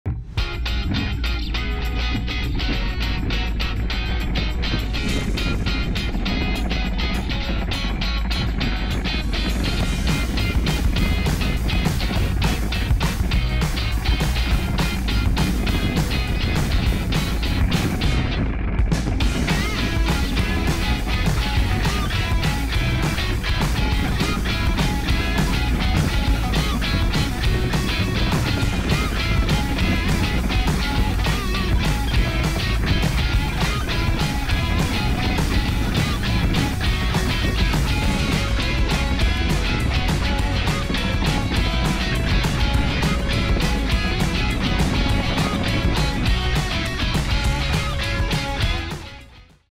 Pipeline: The Surf Coaster, opening at SeaWorld Orlando May 27th!
5 Positive Stars Pipeline: The Surf Coaster, opening at SeaWorld Orlando May 27th! Prepare to catch the ultimate wave at SeaWorld Orlando.